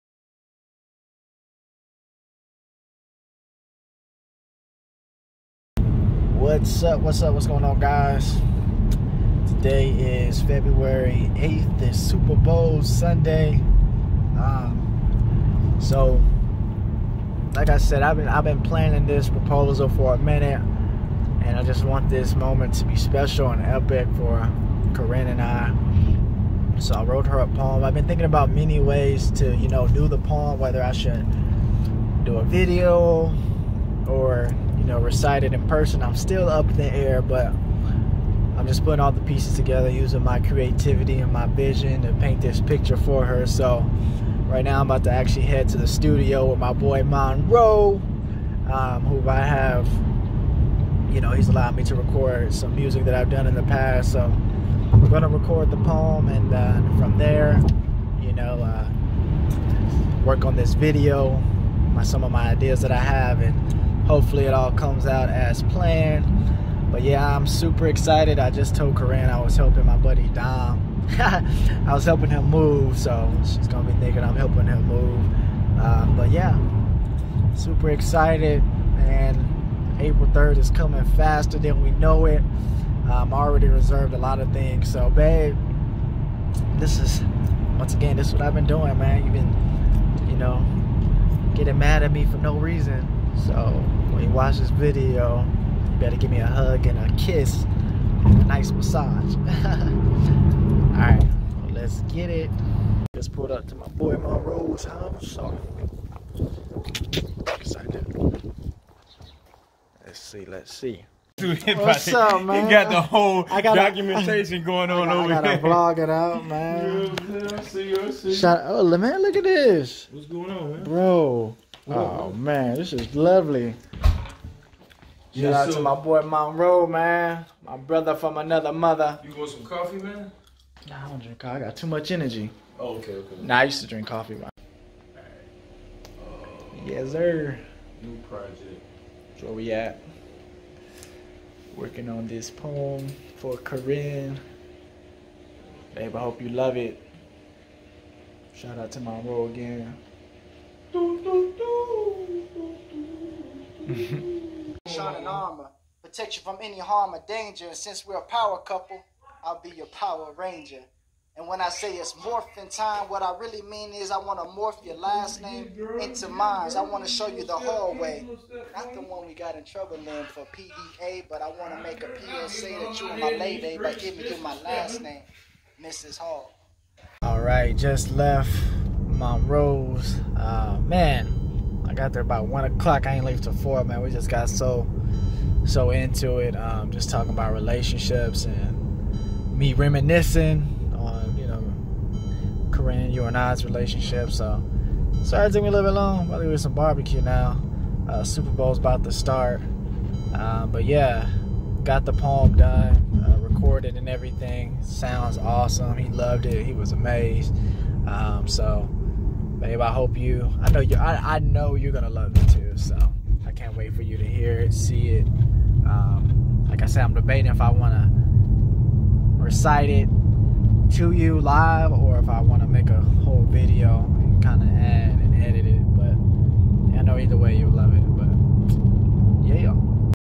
Planning The Perfect Proposal | Part 5: Poetry In Motion
Part 5 of "Planning The Perfect Proposal" I wrote a poem for her! I wanted this moment to be very special every moment had a purpose to it! I use to write poems to her all the time growing up. I wrote and recorded this one I call "Hear My Heart"
Follow of on IG: @theofficiallaroyce